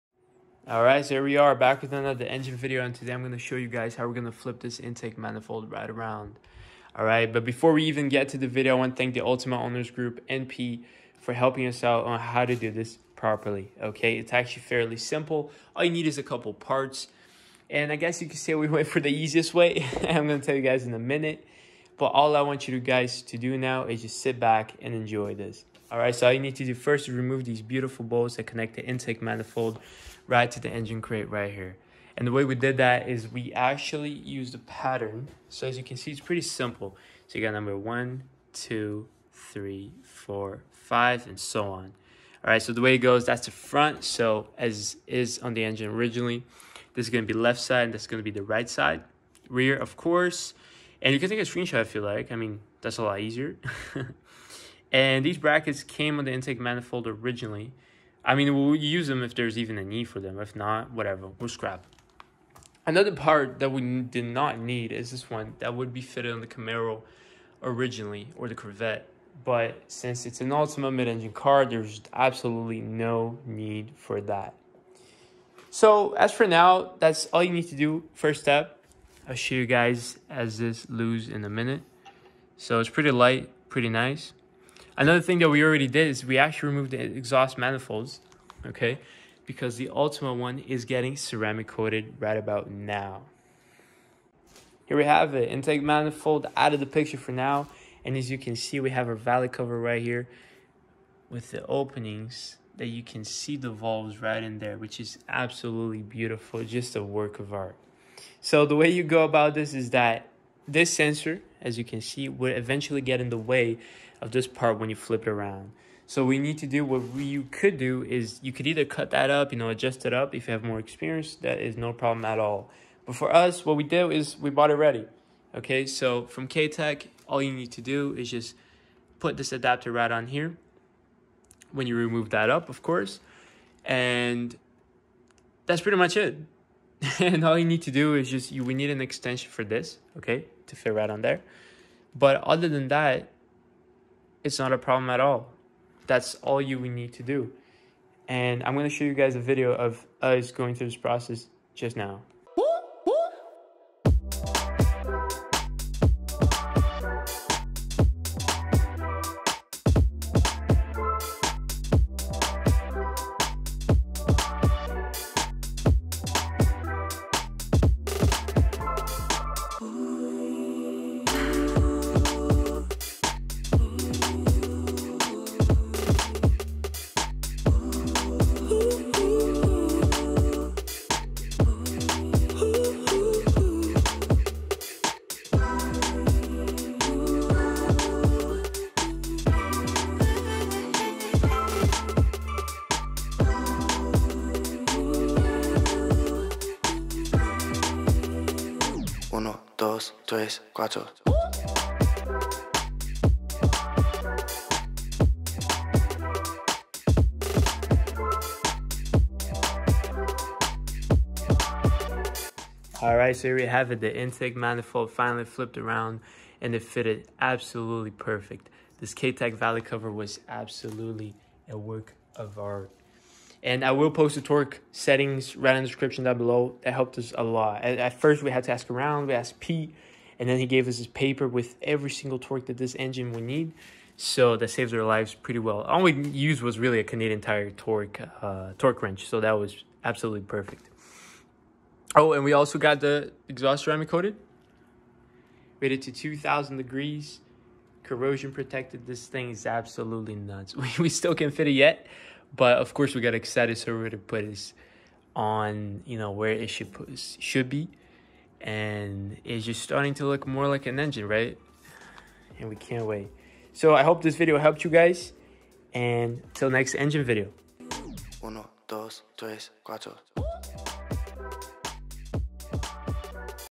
LS3 - Inverting the Intake Manifold
Based on the Katech Valley Cover, while using the GM Intake Manifold and our skills, plus some great help, we are flipping it around for a proper fit on the Mid Engine Layout of the Ultima.